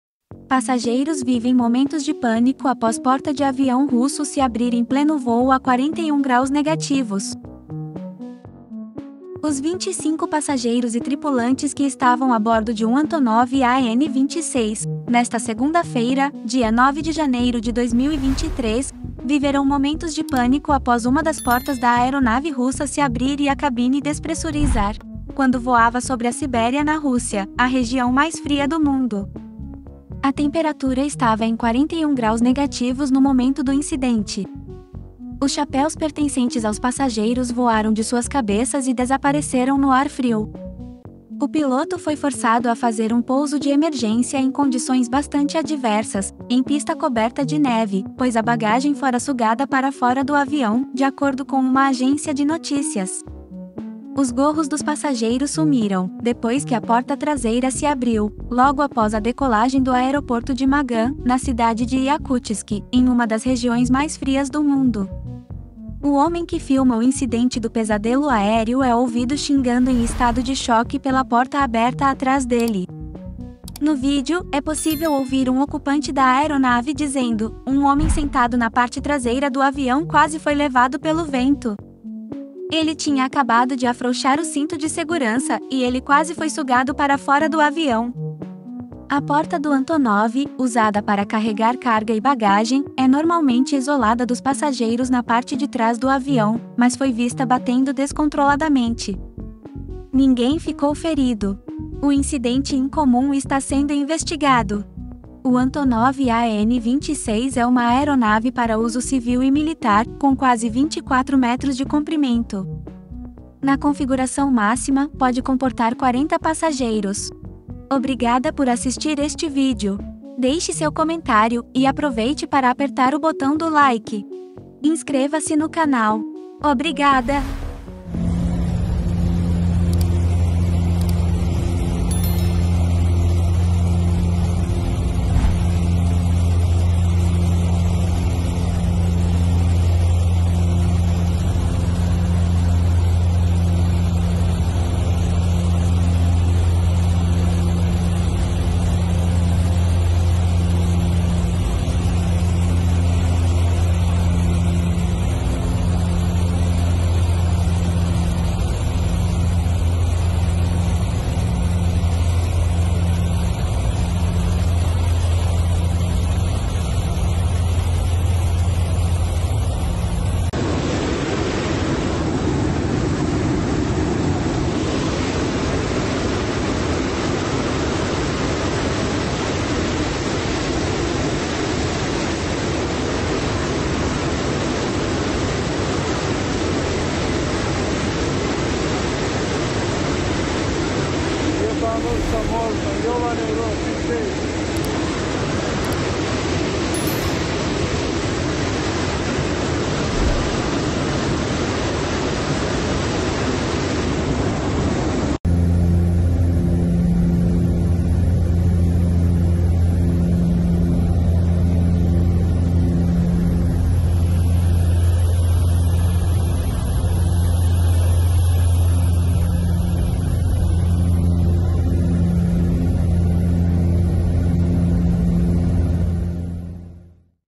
Porta de avião russo Antonov AN-26 se abre durante voo e assusta passageiros
Passageiros vivem momentos de pânico após porta de avião russo se abrir em pleno voo a 41 graus negativos.

Os 25 passageiros e tripulantes que estavam a bordo de um Antonov AN-26, nesta segunda-feira, dia 9 de janeiro de 2023, viveram momentos de pânico após uma das portas da aeronave russa se abrir e a cabine despressurizar, quando voava sobre a Sibéria, na Rússia, a região mais fria do mundo. A temperatura estava em 41 graus negativos no momento do incidente. Os chapéus pertencentes aos passageiros voaram de suas cabeças e desapareceram no ar frio.

O piloto foi forçado a fazer um pouso de emergência em condições bastante adversas, em pista coberta de neve, pois a bagagem fora sugada para fora do avião, de acordo com uma agência de notícias.

Os gorros dos passageiros sumiram, depois que a porta traseira se abriu, logo após a decolagem do aeroporto de Magan, na cidade de Yakutsk, em uma das regiões mais frias do mundo.